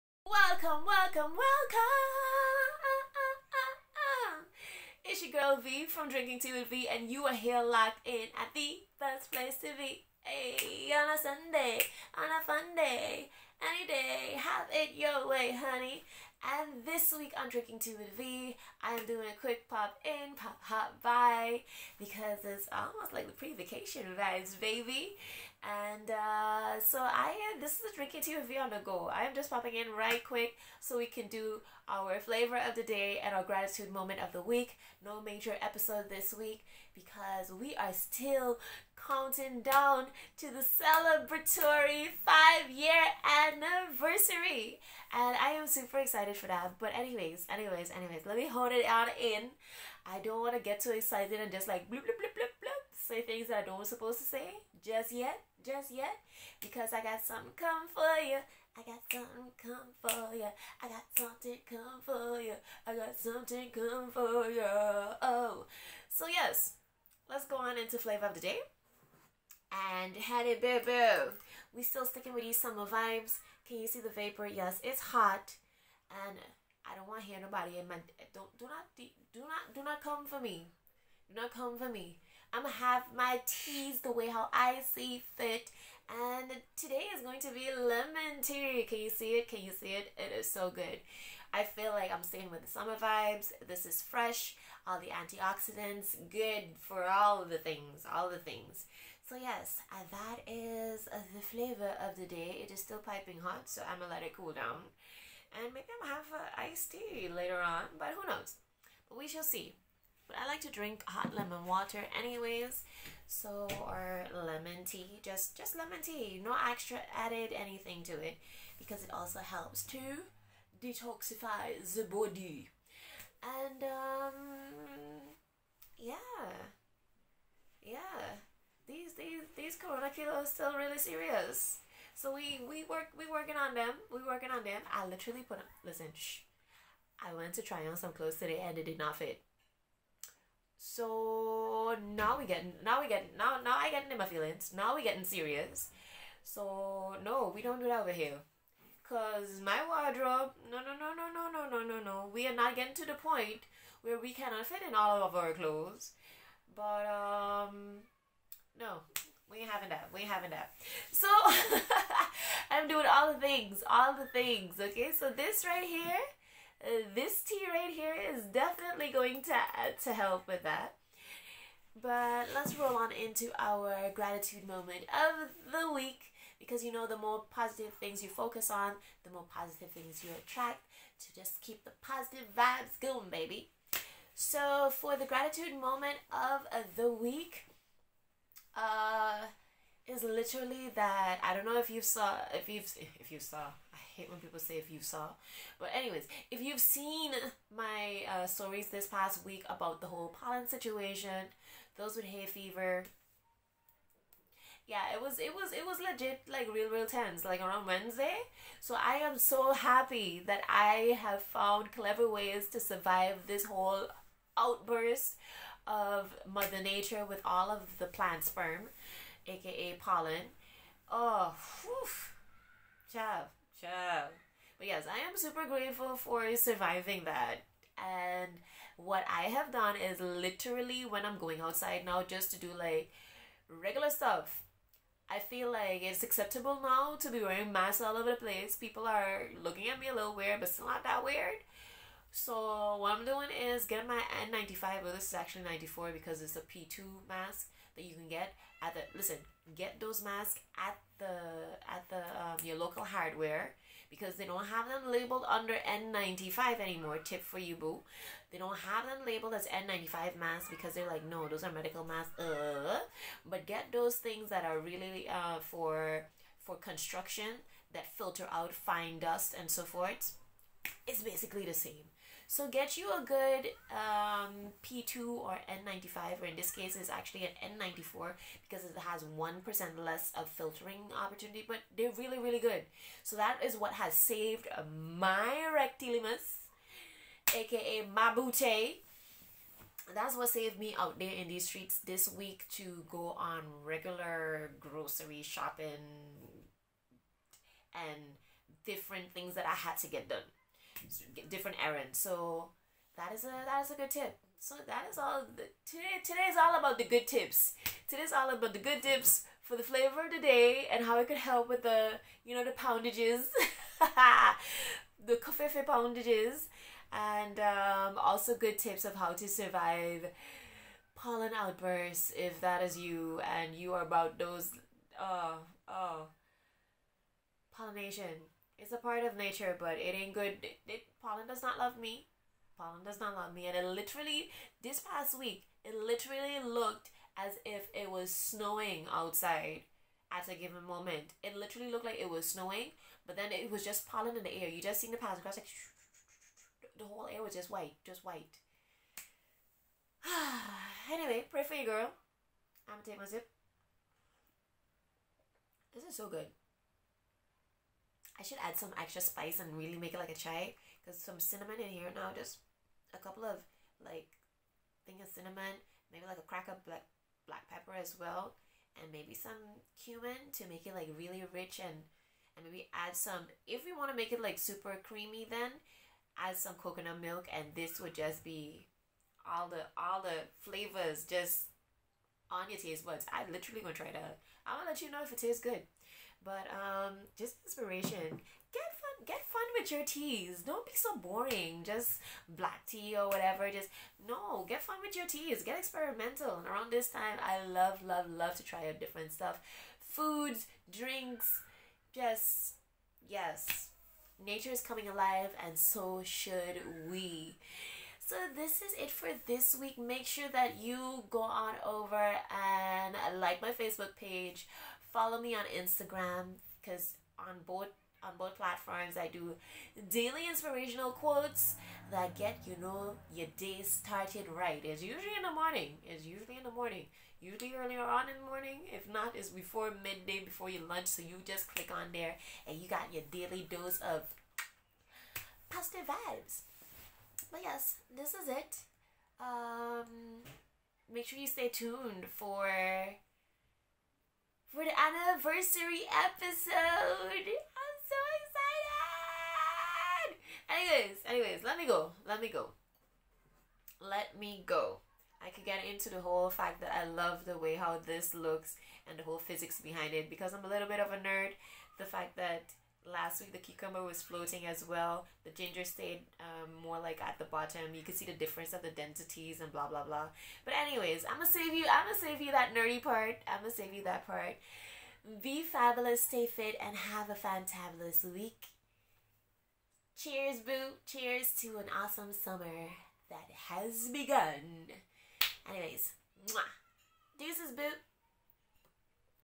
DTWV Eps - Tummy Trimming and Hay Fever tips - DTWV on the go ✌️
Dealing with allergies can be a pain in the butt! If you can't beat it, you need to be creative to come up with solutions. So check out this episode and let me know what you think about these tips in the comments below!

(for blogs and more inside scoop - all the tea, honey, all the tea!)